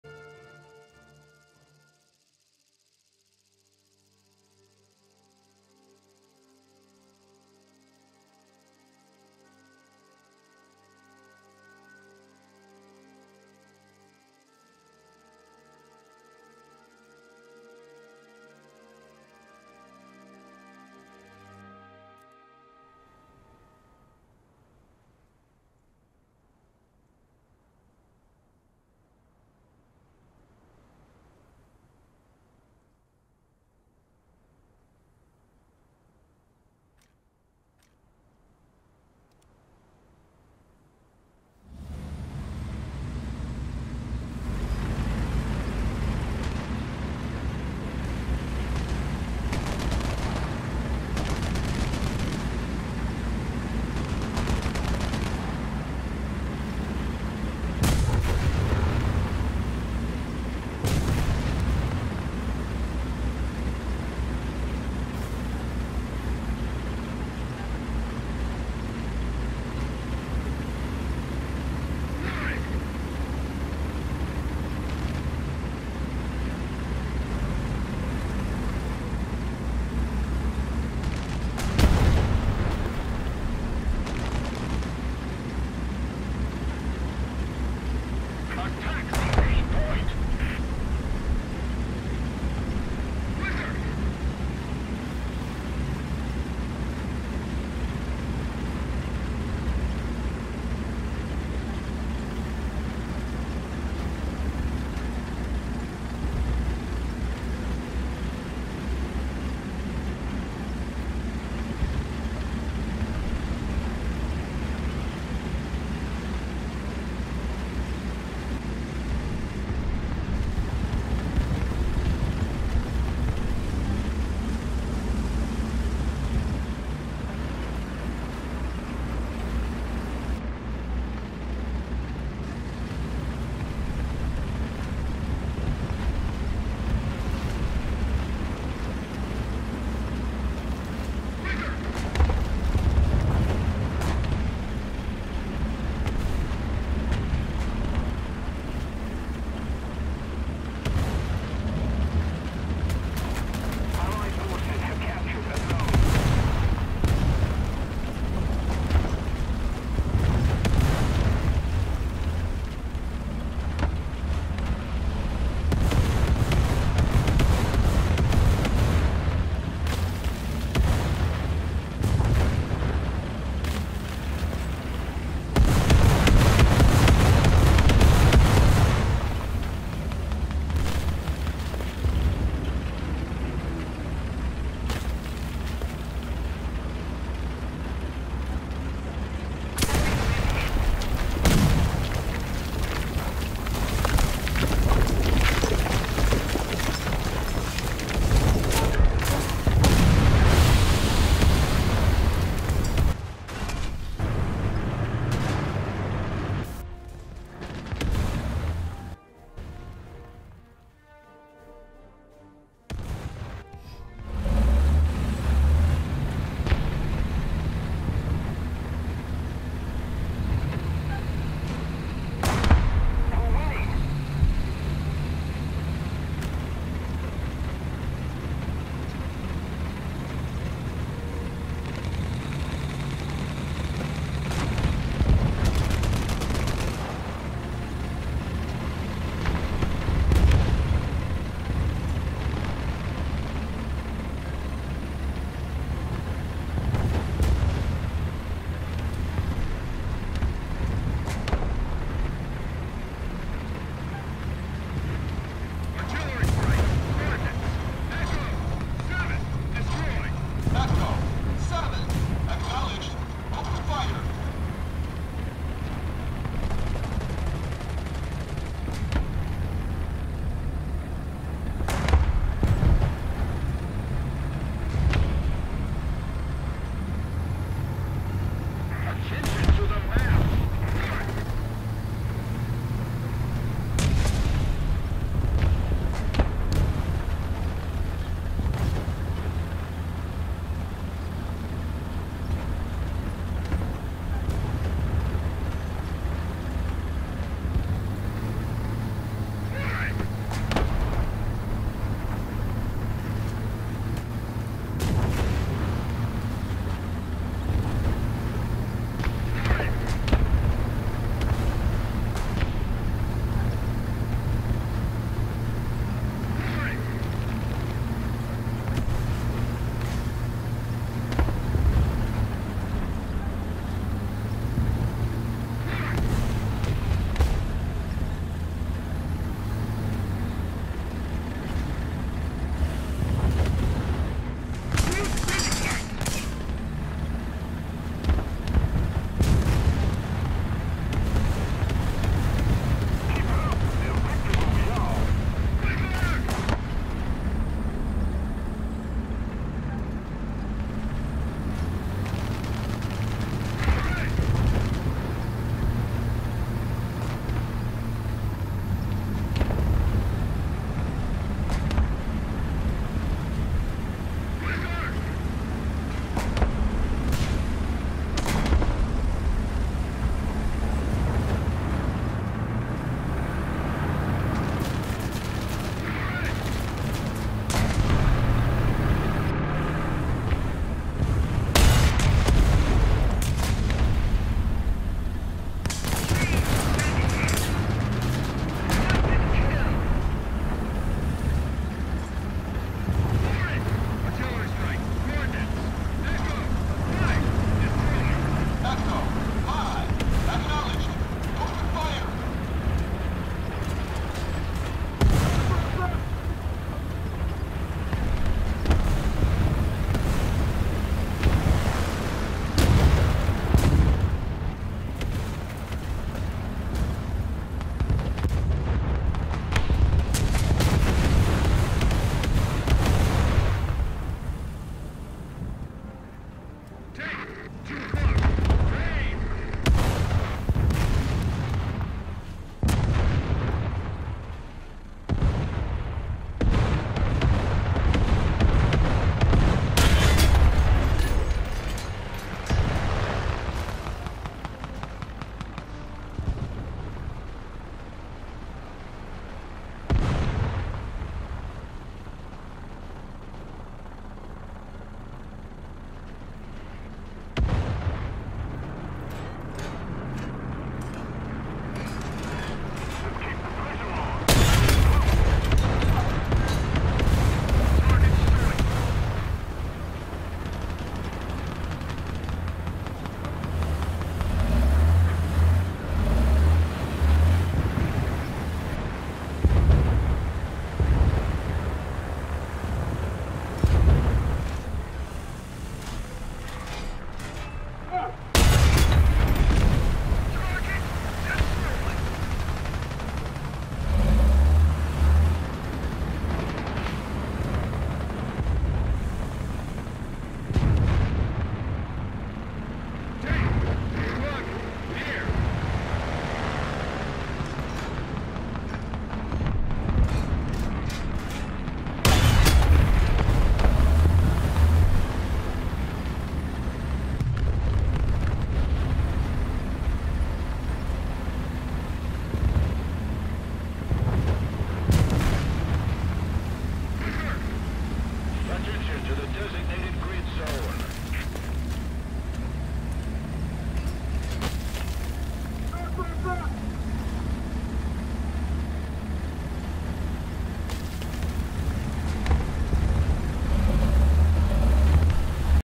Abusing two IS-6's with the CEV with some of me getting obliterated
Cause when the CEV shows up to the fight, ain’t no one safe. Not even IS-6’s